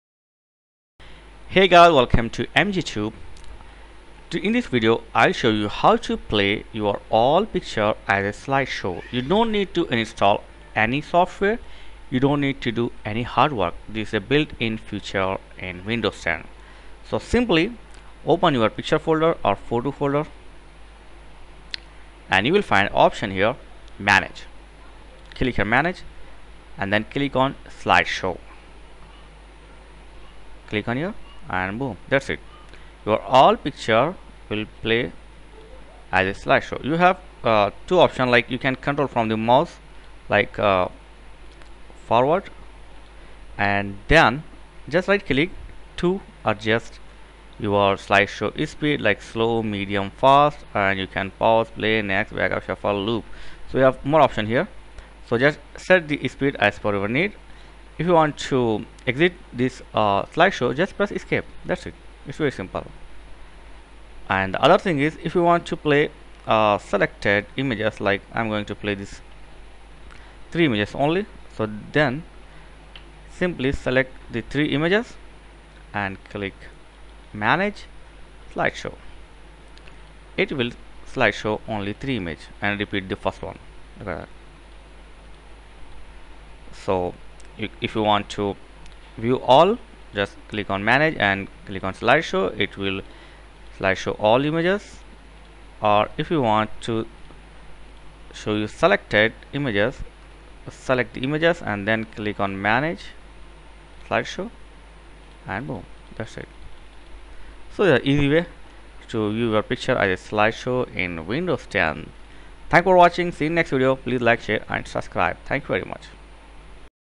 How to View Pictures as a Slideshow Without any Software
There is a in-built feature in Windows 10 to view all pictures as a slideshow, you don’t need to use any software and don’t need to do any hard work,